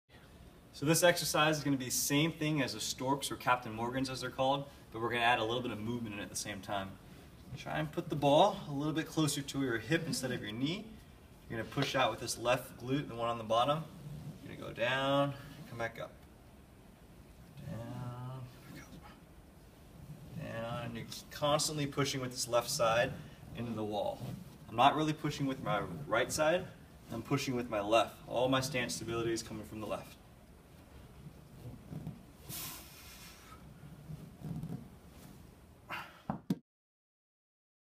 Running Storks
We're big advocates of the stork, or Captain Morgan, exercise for GLUTE STRENGTHENING. Once you can properly perform the stork, try implementing a little bit of sport specific movement into the exercise!

Here, I'm trying my best to replicate the biomechanics at the hip for a runner. The glutes are our primary controller of frontal plane motion in running. During loading response (when you strike the ground), your glutes need to eccentrically control your femur moving into flexion. As you progress over your foot, it requires the glutes to concentrically move the femur into extension. I added a more forceful concentric phase here to again, try my best to replicate the demands on the hips during running.